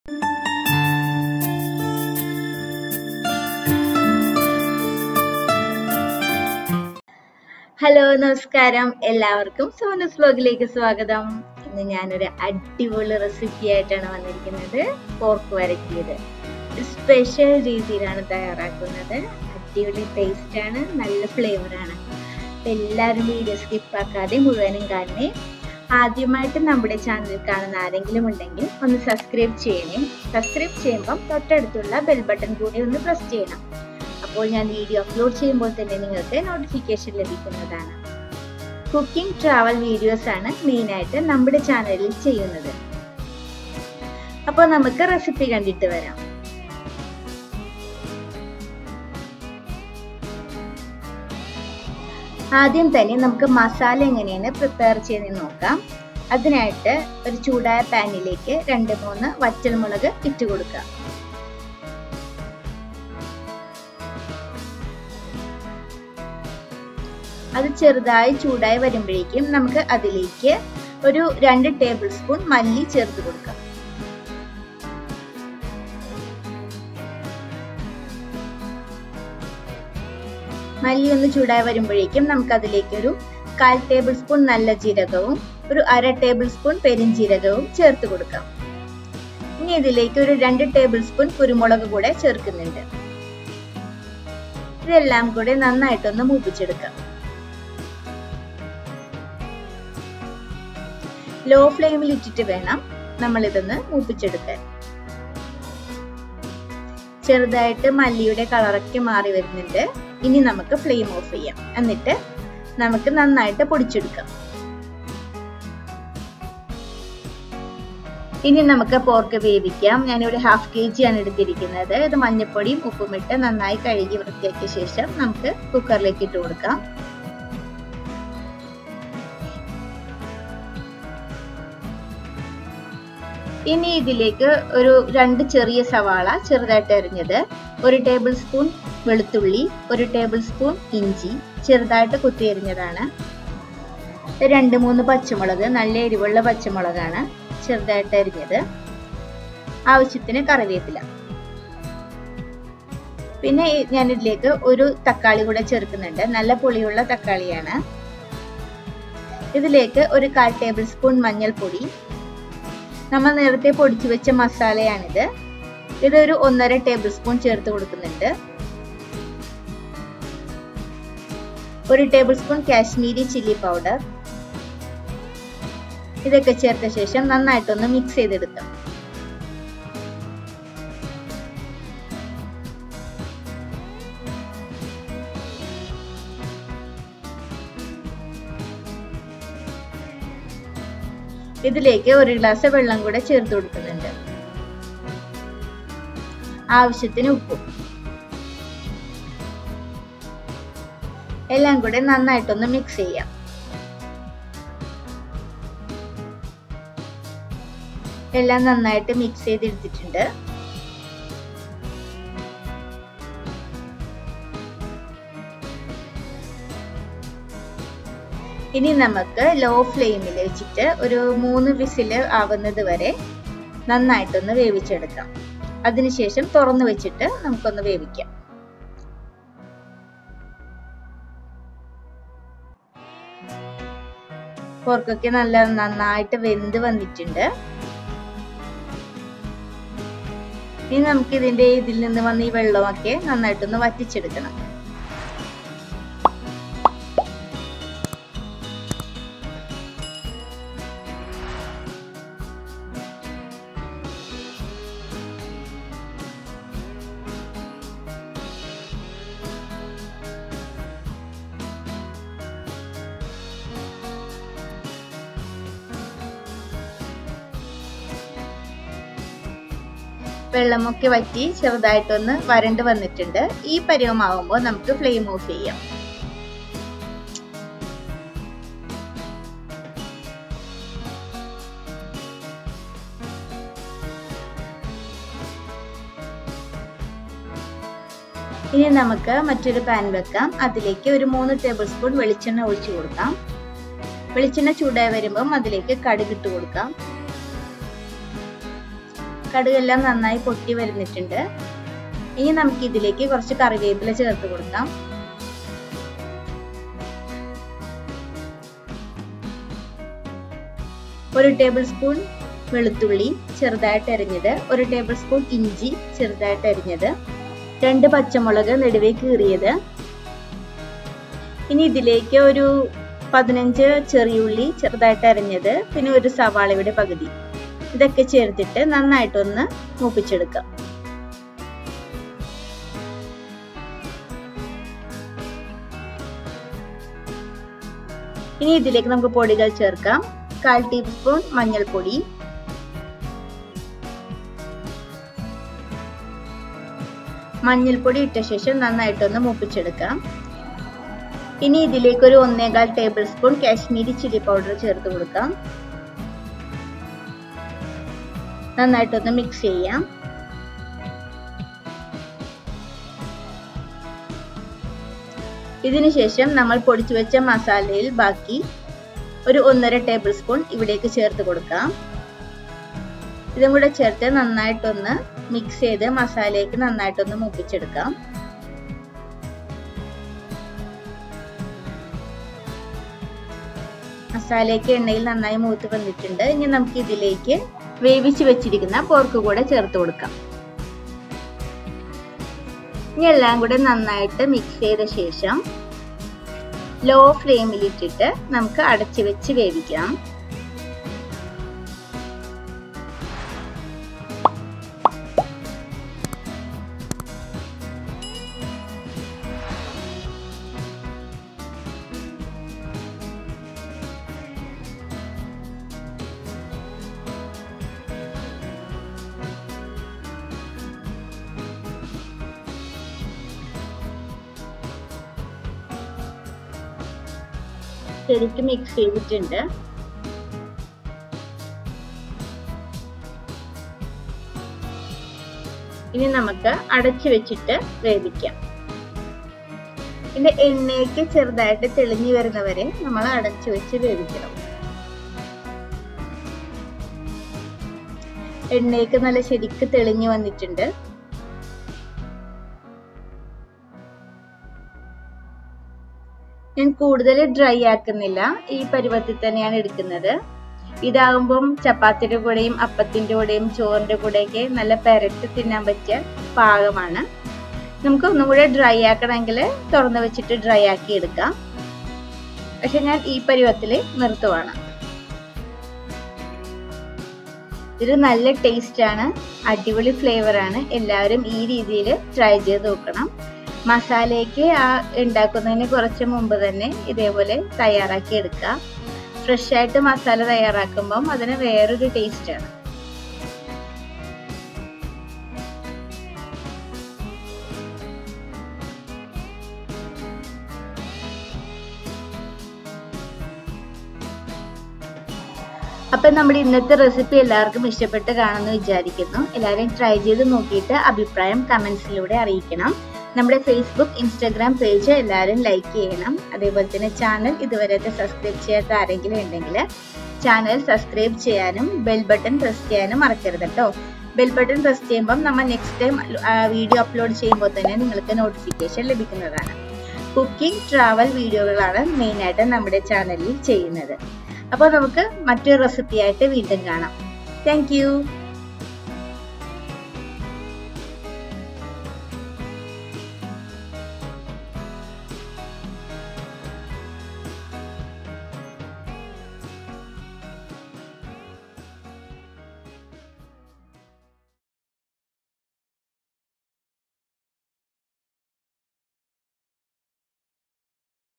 കിടിലൻ പോർക്ക്‌ റെസിപ്പി | Pork Recipe | Kerala style pork varattiyathu | Sonus Vlog
Ingredients

Step – 1

Dry Chili – 4
Coriander – 2 Table Spoon
Cumin Seed – ¼ Table Spoon
Sweet Fennel – ½ Table Spoon
Pepper – 2 Table Spoon

Step – 2

Pork – ½ Kg
Onion – 2 Small Size
Garlic – 1 Table Spoon
Ginger – 1 Table Spoon
Green Chili – 2
Curry Leaves
Tomato - 1
Turmeric Powder – ¼ Table Spoon
Masala – 1 ½ Table Spoon
Kashmiri Chili Powder – 1 Table Spoon
Water
Salt

Step – 3

Coconut Oil – 3 Table Spoon
Mustard
Curry Leaves
Chili – 2
Small Onion – 15
Onion – ½
Turmeric – ¼ Table Spoon
Kashmiri Chili Powder – 1 ¼ Table Spoon

Thanks & Regards,
Sonus Vlog